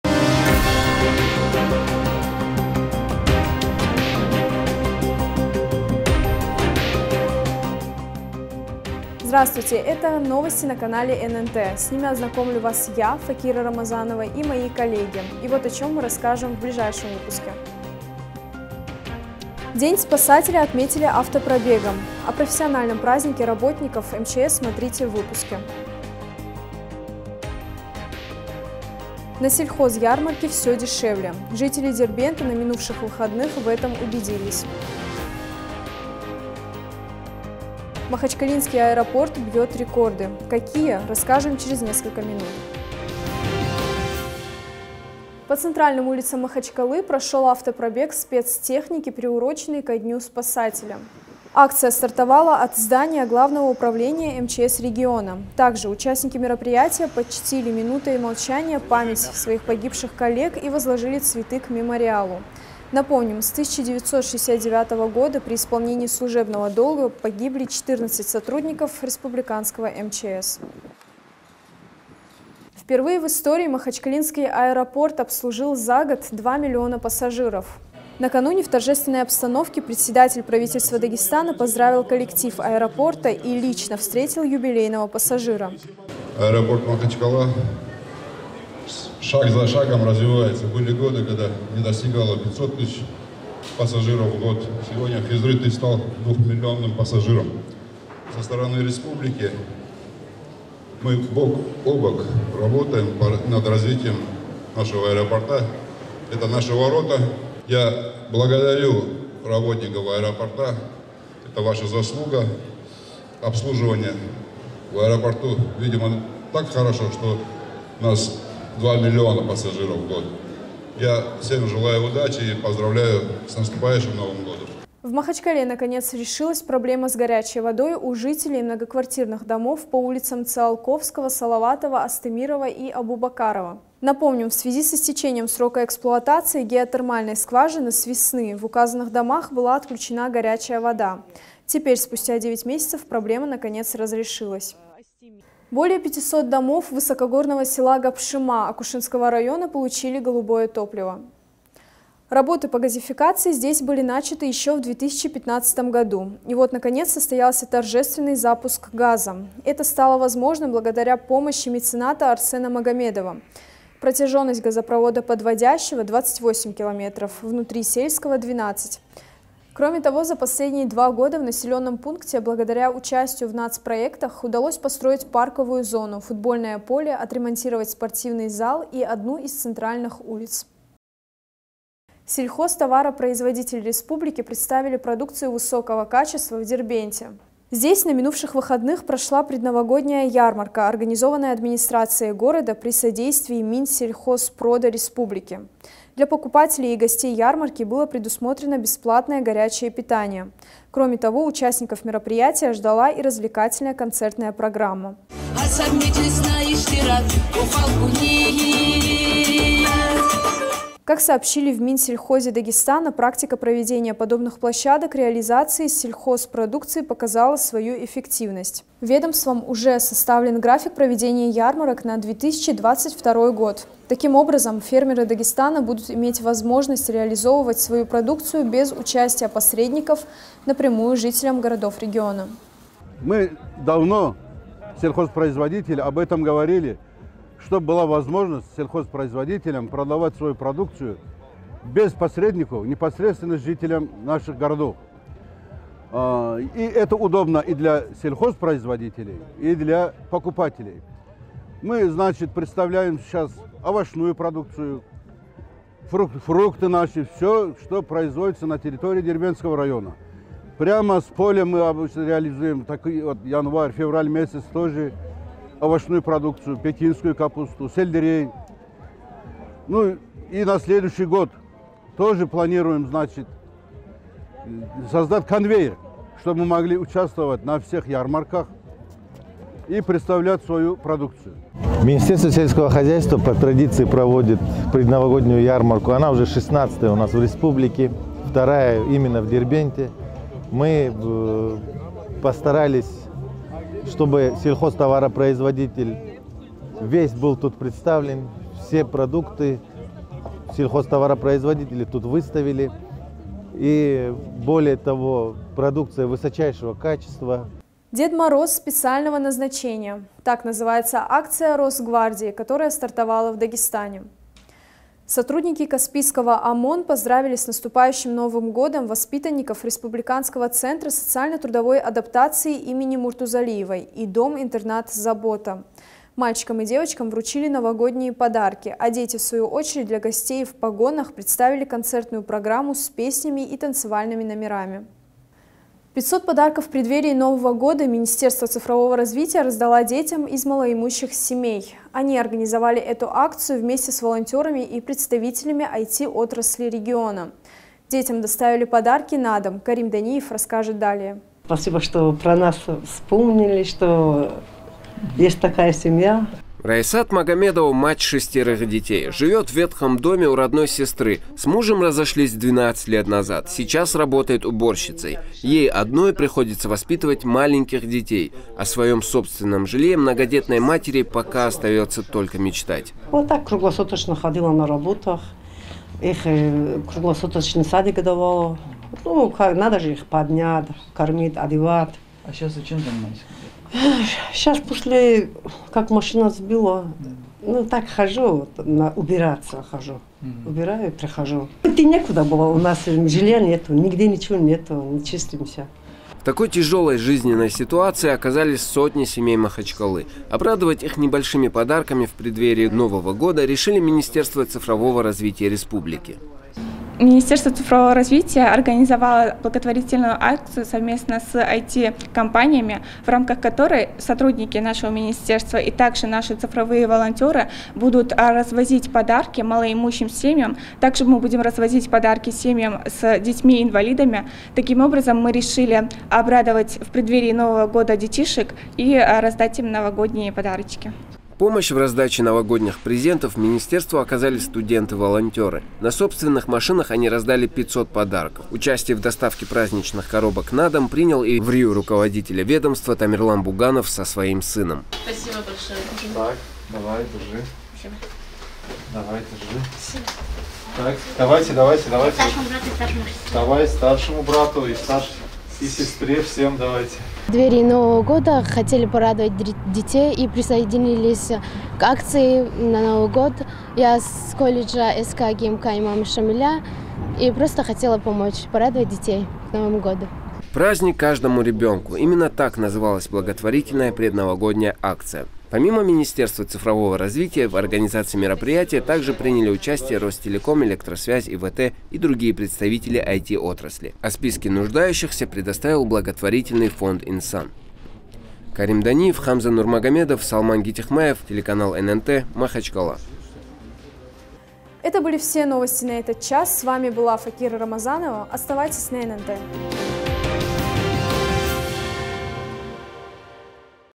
Новости Дагестана | Обеденный выпуск за 28.12.2021 года
1. День спасателя отметили автопробегом. О профессиональном празднике МЧС смотрите в выпуске.
2. На Сельхозярмарке все дешевле. Жители Дербента на минувших выходных в этом убедились.
3. Махачкалинский Аэропорт бьет рекорды. Какие расскажем сегодня.